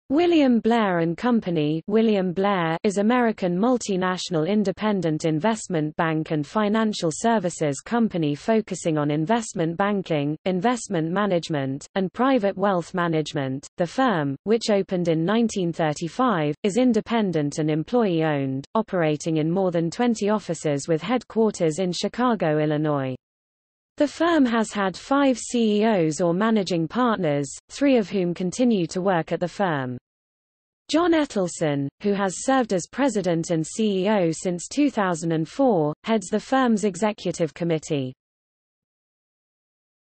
William Blair & Company | Wikipedia audio article
- Socrates

SUMMARY
William Blair & Company ("William Blair") is American multinational independent investment bank and financial services company focusing on investment banking, investment management, and private wealth management.The firm, which opened in 1935, is independent and employee-owned, operating in more than 20 offices with headquarters in Chicago, IL. The firm has had five CEOs or managing partners, three of whom continue to work at the firm. John Ettelson, who has served as president and CEO since 2004, heads the firm's executive committee.